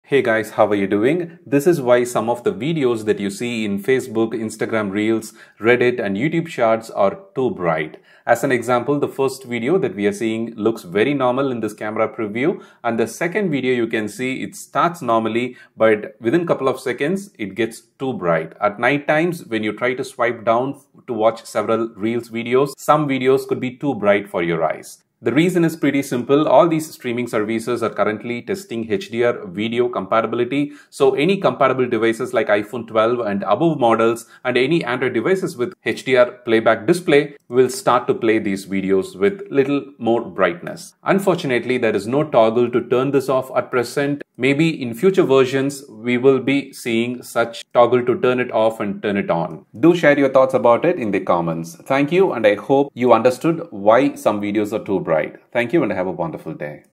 Instagram Reels Videos Too Bright, 🌞 Why?
Instagram Reels Videos Too Bright? Not Just Instagram, also some Facebook Videos, Reddit Videos and Youtube Shorts too Bright in iPhone. Here is Why it's too Bright? and sadly it's not possible to Turn it OFF right now.